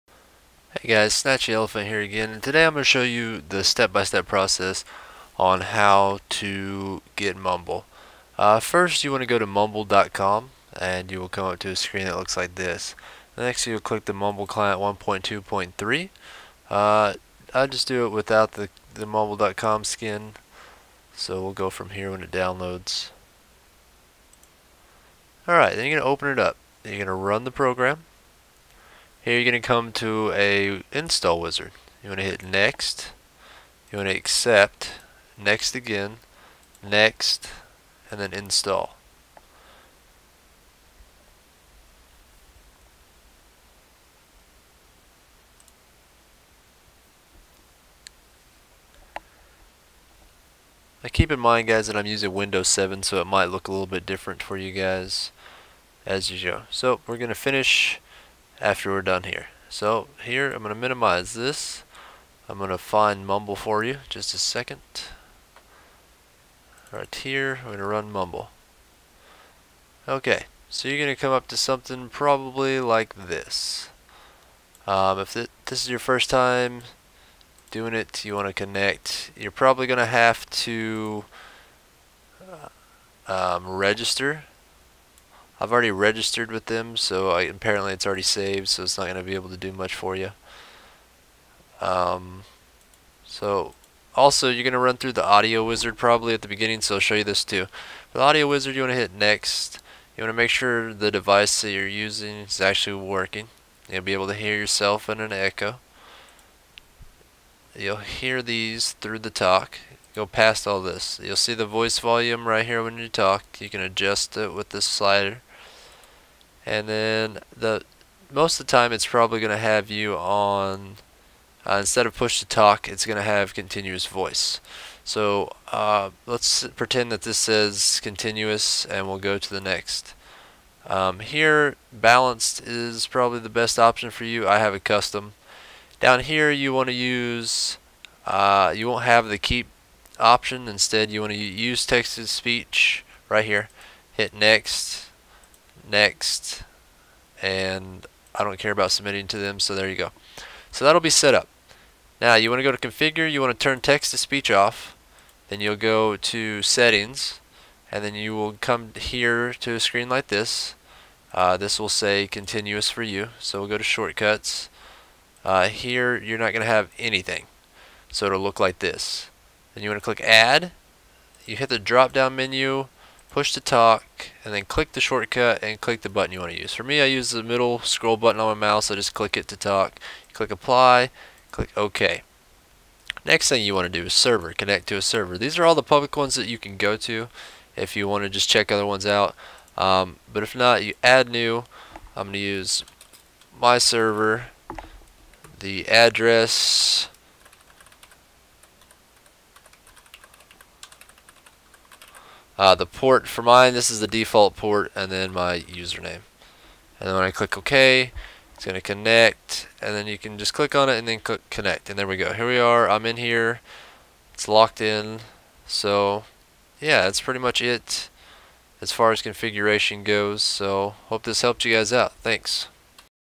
How to Mumble (Tutorial)
A little tutorial that covers a little bit more in depth on how to setup mumble. hope you enjoy